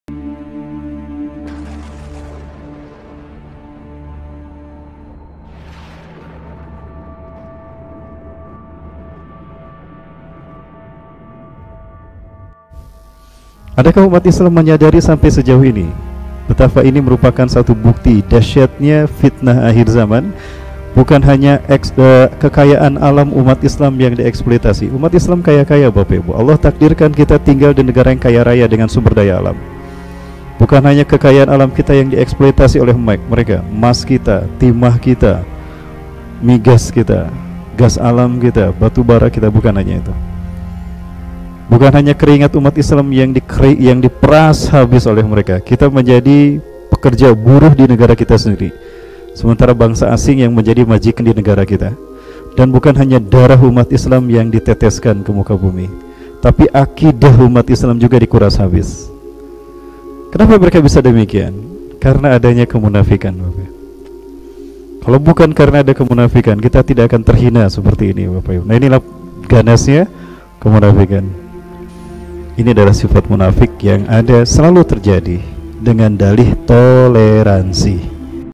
RENUNGAN SINGKAT USTADZ RAHMAT BAEQUNI UNTUK UMAT ISLAM INDONESIA
Ini adalah renungan singkat ustadz rahmat baequni,,
Ustadz kami doakan semoga engkau selalu dijaga Allah SWT dan selalu diberi kesehatan agar senantiasa memberikan semangat kepada kami rakyat indonesia